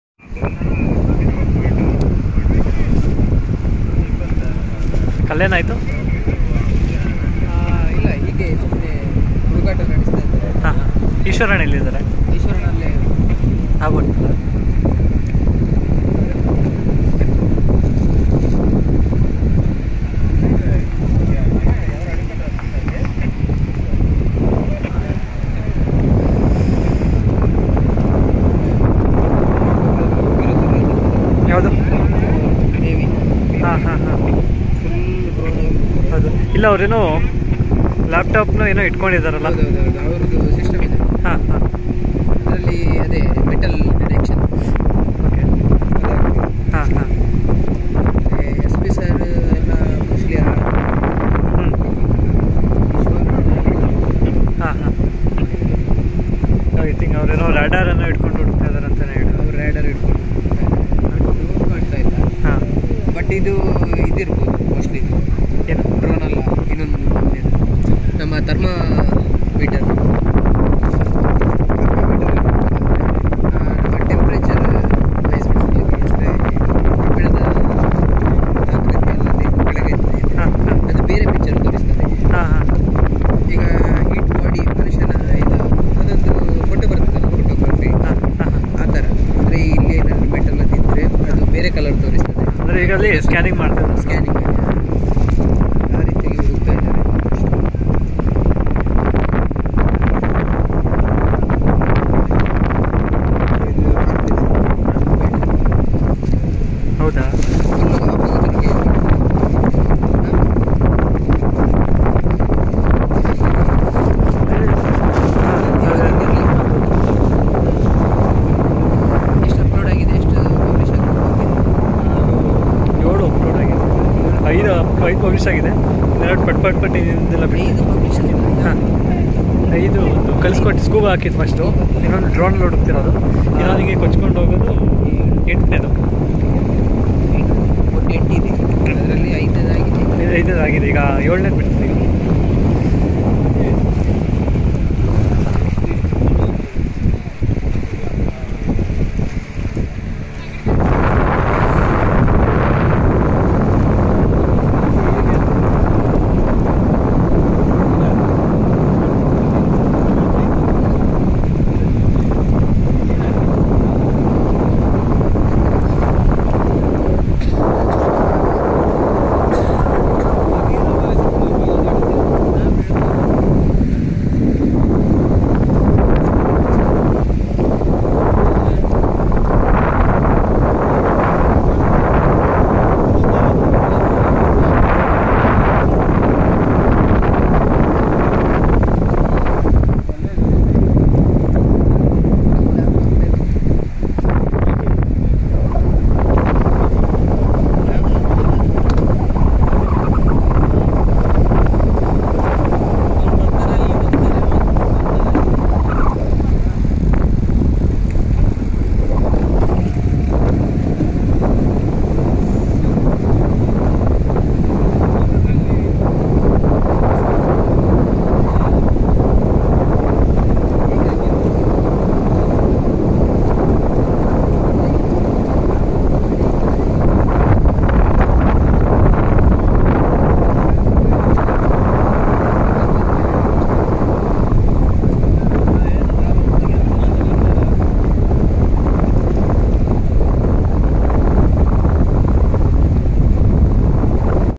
Laptop ಮೂಲಕ ಹೊಸ ತಂತ್ರಜ್ಞಾನ ಬಳಸಿ "ಅರ್ಜುನ್"ಗಾಗಿ ಹುಡುಕಾಟ..!! | Live from Ankola
ಕರ್ನಾಟಕದ ಎಲ್ಲಾ ಕಡೆಯಲ್ಲಿಯೂ ಉಚಿತ ಸೇವೆಯನ್ನು ನೀಡುತ್ತಿರುವ ಈಶ್ವರ್ ಮಲ್ಪೆ ಅವರ ಚಾನೆಲ್ ಸಬ್ಸ್ಕ್ರೈಬ್ ಮಾಡಿ ಹಾಗೂ ಸಪೋರ್ಟ್ ಮಾಡಿ.

Eshwar Malpe 24×7 Social activities